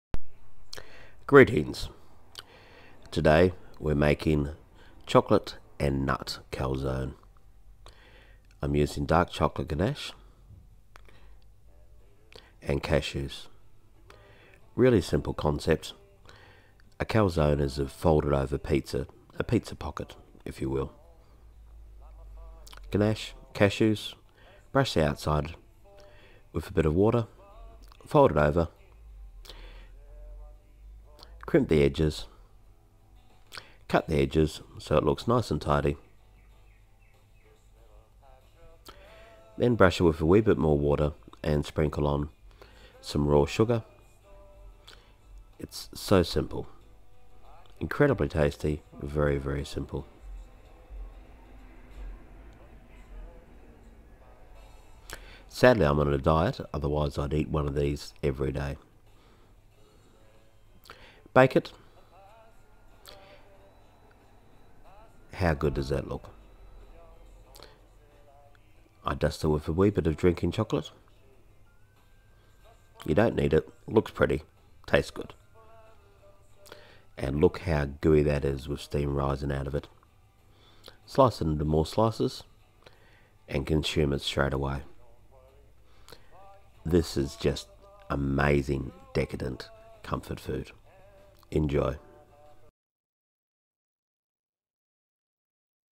Chocolate & Cashew Calzone
Gooey chocolate goodness. Simple concept, easy to make and very decadent, this is a dish that one can easily fall in love with.